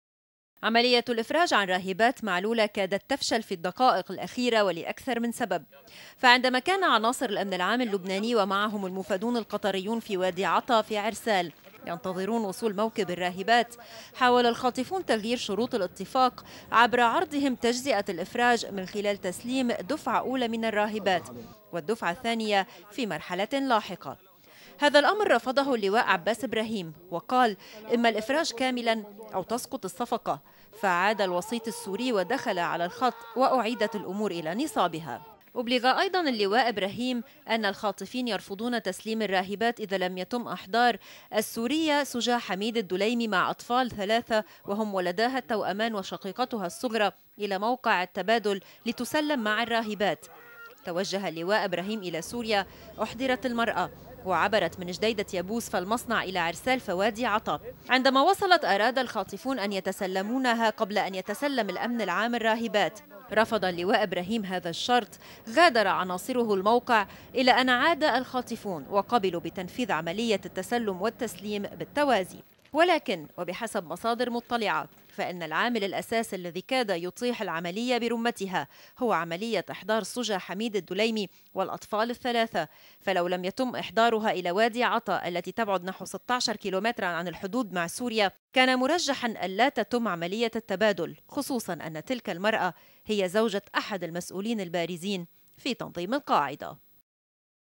LBCI News-من هي سجى الدليمي التي كادت تودي بعملية الافراج عن الراهبات ؟
انهاء عملية الافراج عن الراهبات كاد يفشل في الدقائق الاخيرة ولاكثر من سبب...